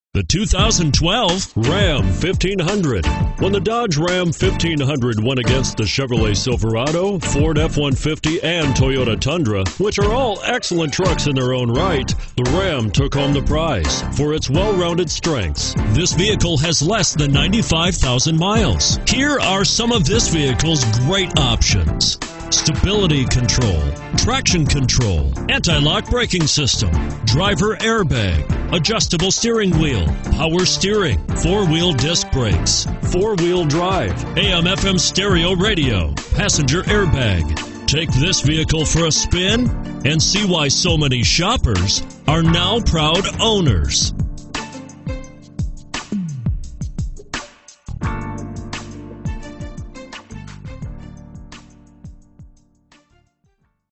2012 Ram 1500 Tyler TX 15HA268A
2600 W SW Loop 323

Pick up this Flame Red 2012 Ram 1500, available today at Jack O'Diamonds Honda-Lincoln. Featuring Automatic transmission, it has 94365 miles. This could be the one you've been searching for.